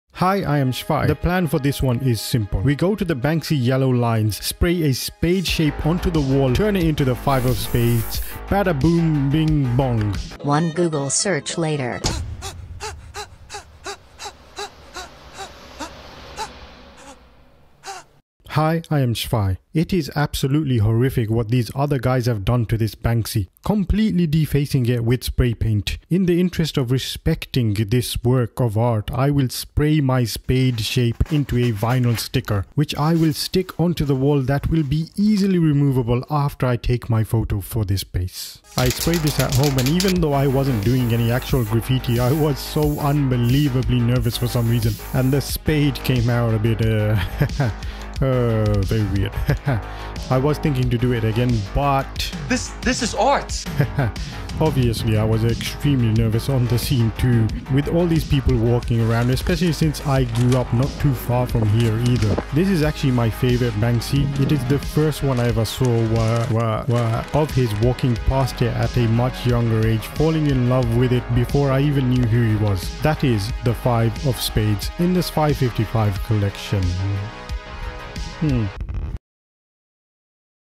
Defacing a Banksy for an NFT | XPHY55.06 Five of Spades
Making of the NFT Digital Art XPHY55.06 Two of Diamonds

Xphy Character mask based on Wintercroft Masks:
Horned Barbarian Helmet